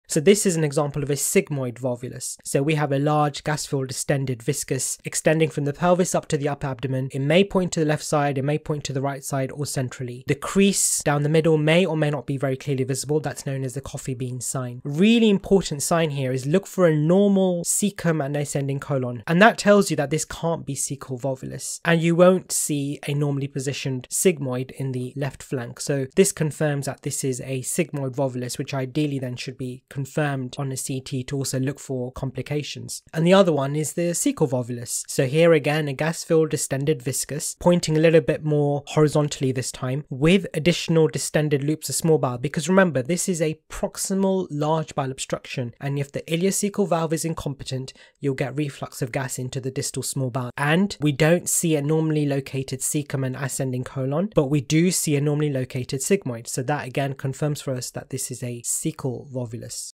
Identifying sigmoid volvulus on abdominal x-ray
Clip from Radiopaedia's abdominal x-ray lecture by Dr Vikas Shah.